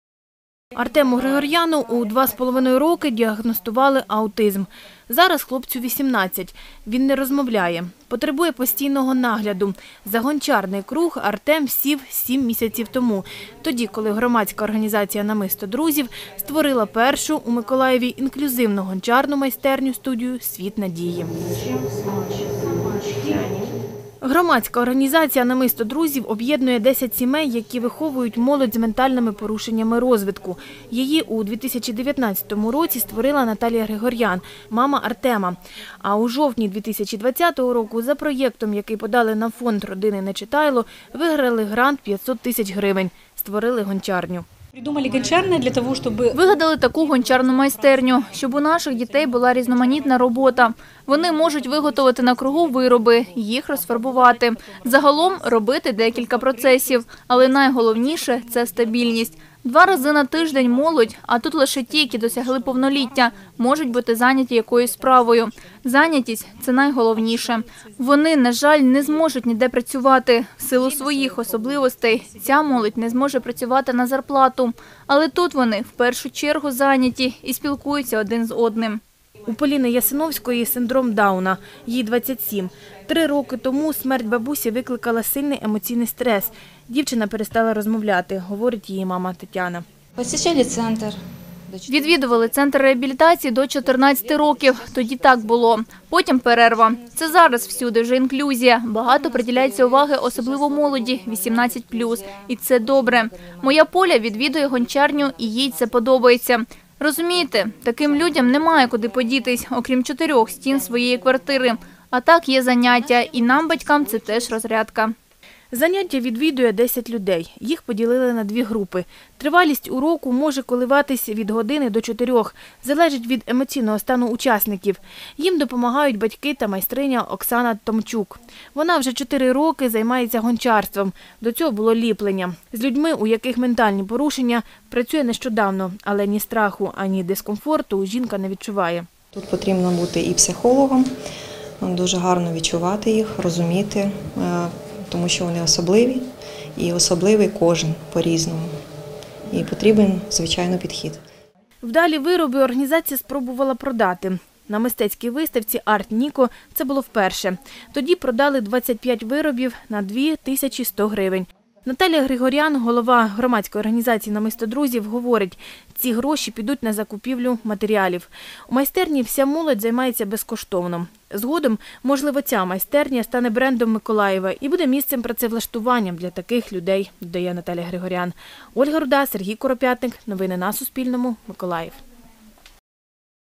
Для молоді з ментальними порушеннями розвитку в Миколаєві створили гончарню
У Миколаєві діє перша гончарня для молоді з ментальними порушеннями розвитку. ЇЇ відкрила громадська організація "Намисто друзів". Створені вироби продають.
Авторка - Ольга Руда, оператор - Сергій Куропятник.